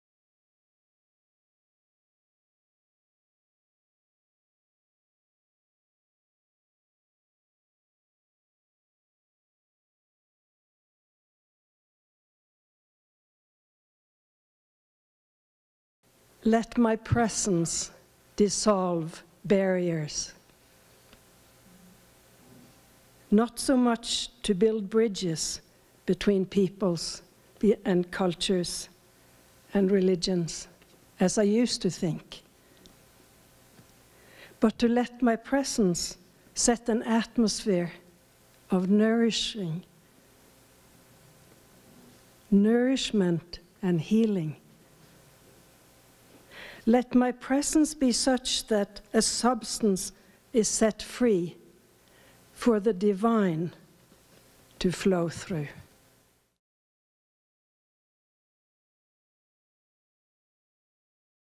Kari Bye Reads an Original Poem
October 8, 2017: EDL Server Kari Bye reads an original poem called "Presence" during the Sunday service in the Dome at Sunrise Ranch, Loveland, Colorado.

SunriseRanch.org